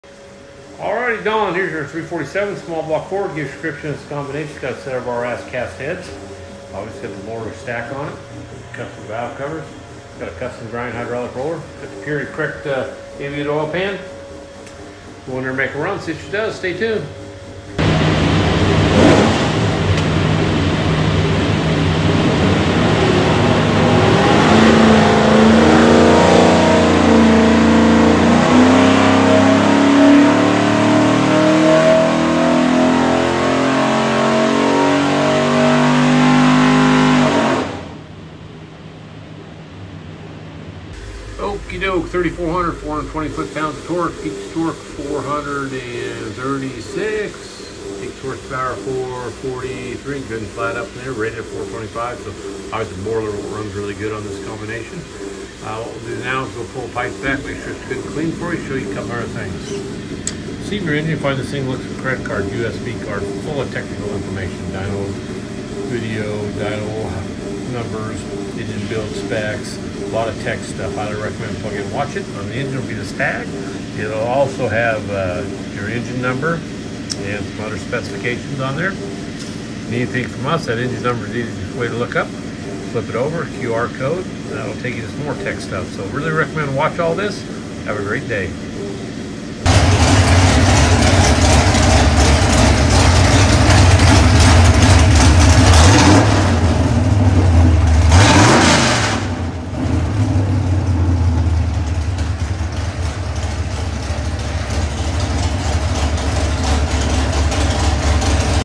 444HP Stacked 347 Stroker Dyno Test for Don's Race Car Replicas GT40 at Prestige Motorsports
444HP Stacked 347 Stroker Dyno Test for Don's Race Car Replicas GT40 at Prestige Motorsports

Prestige Motorsports As Cast heads
Borla Stack Injection
Holley Terminator X MPEFI
COMP CAMS custom grind hydrualic roller
Aviaid oil pan
Shelby Valve Covers